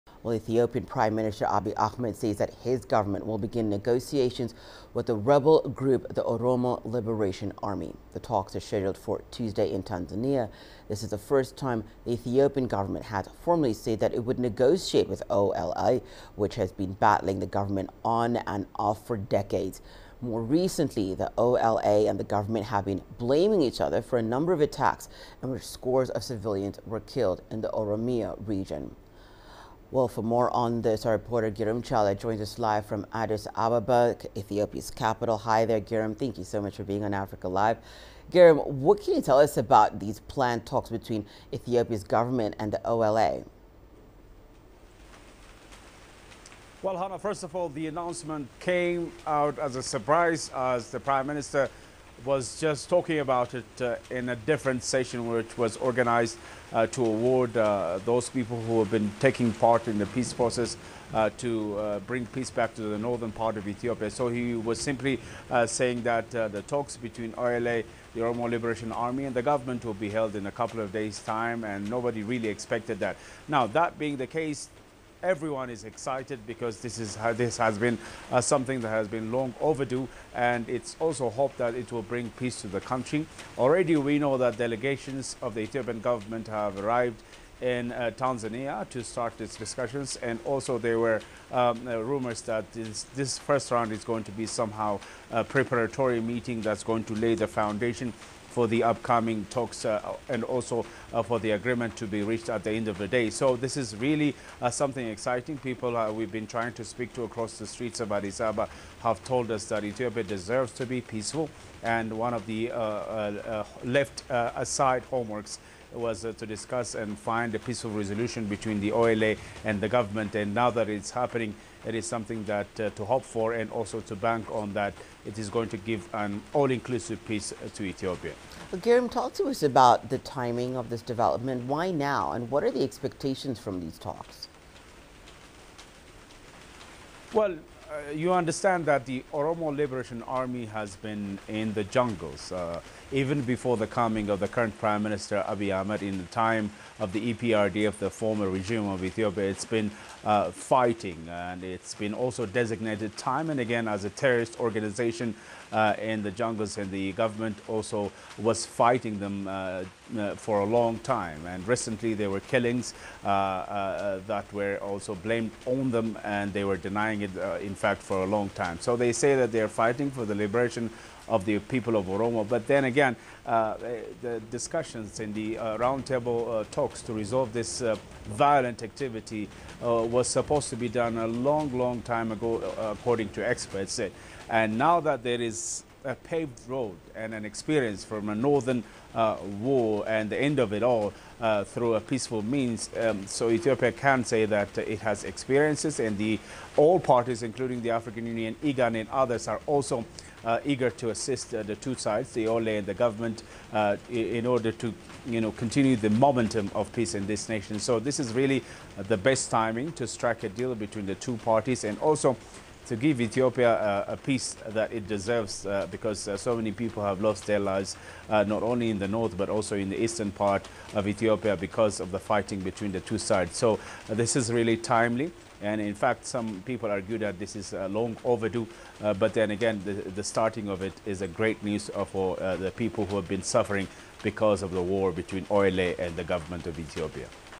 Ethiopian government to begin negotiations with OLA rebel group
Delegations from the Ethiopian federal government are in Tanzania ahead of the start of negotiations with the Oromo Liberation Army (OLA). The first round of talks are expected to lay a foundation for an agreement to be reached towards establishing lasting peace in the Oromia region.